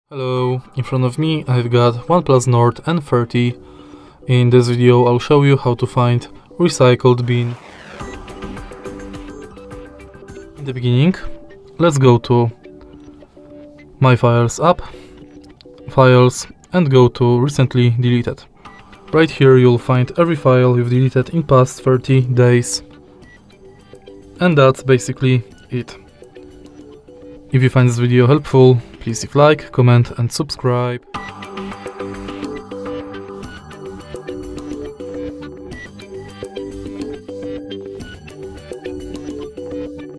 How To Find Recycle BIN On One Plus Nord N30
In this video, we will guide you on how to find the Recycle Bin on your OnePlus Nord N30. The Recycle Bin is a useful feature that allows you to recover deleted files and folders on your device.

Where is the Recycle Bin located on OnePlus Nord N30?
How to access the Recycle Bin on OnePlus Nord N30?
Can I recover deleted files on OnePlus Nord N30?
What happens to deleted files on OnePlus Nord N30?